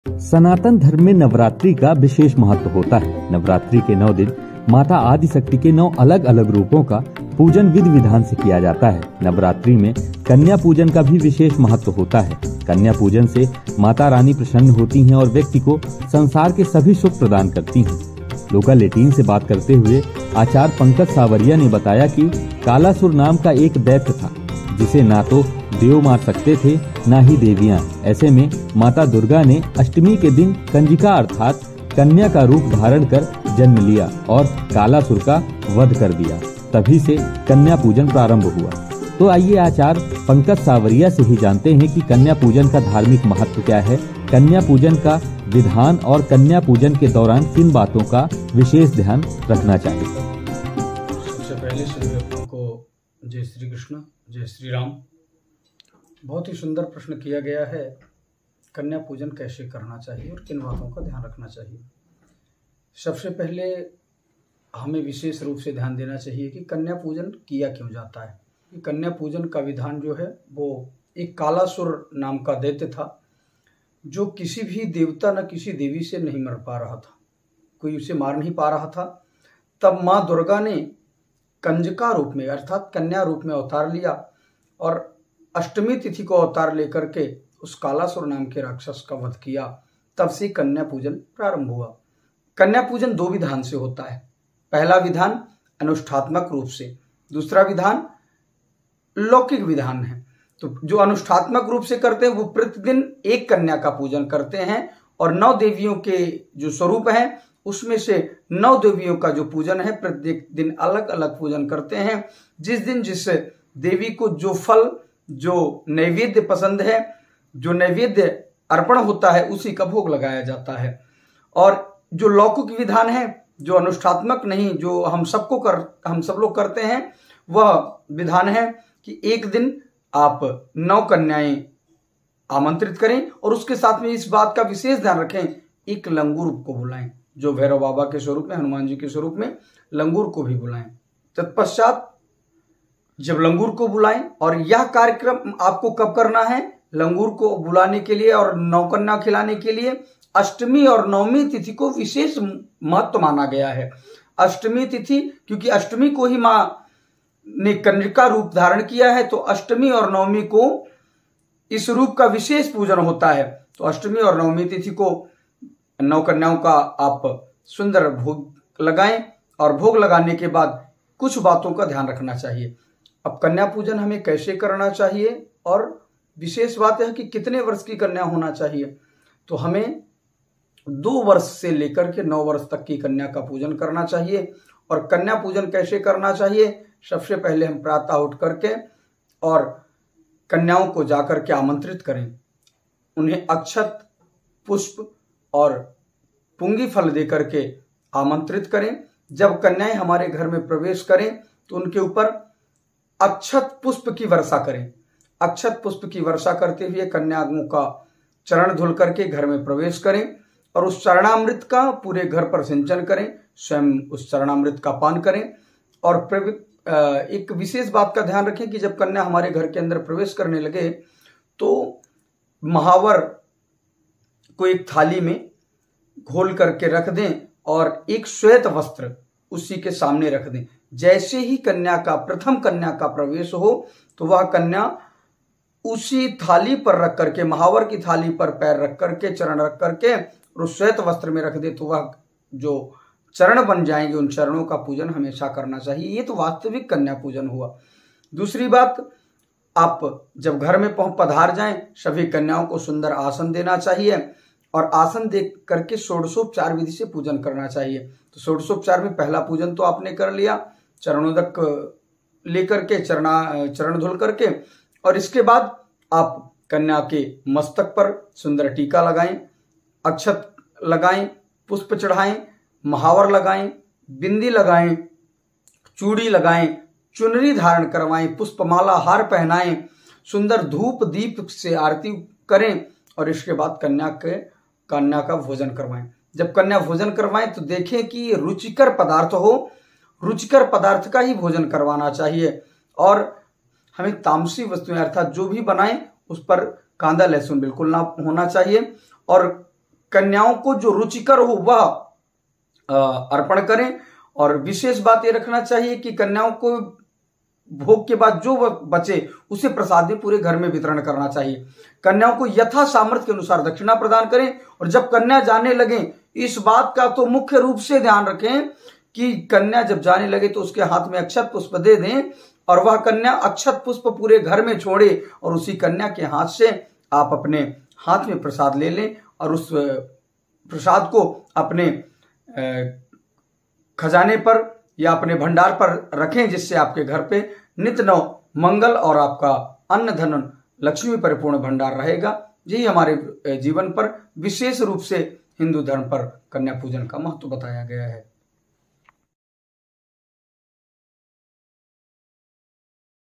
Chaitra Navratri 2024: नवरात्रि में कन्या पूजन के समय इन बातों का खास रखें ध्यान
Navratri has a special significance in Sanatan Dharma. On the nine days of Navratri, nine different forms of Mother Adishakta are worshiped as per the rituals. In Navratri, Kanya Puja also has a special significance. Mata Rani is pleased by Kanya Puja and the person Provides all the happiness of the world,
सनातन धर्म में नवरात्रि का विशेष महत्व होता हैं नवरात्रि के नौ दिन माता आदिशक्त के नौ अलग - अलग रूपों का पूजन विधि विधान से किया जाता है, नवरात्रि में कन्या पूजन का भी विशेष महत्त्व होता हैं कन्या पूजन से माता रानी प्रसन्न होती हैं और व्यक्ति को संसार के सभी सुख प्रदान करती हैं,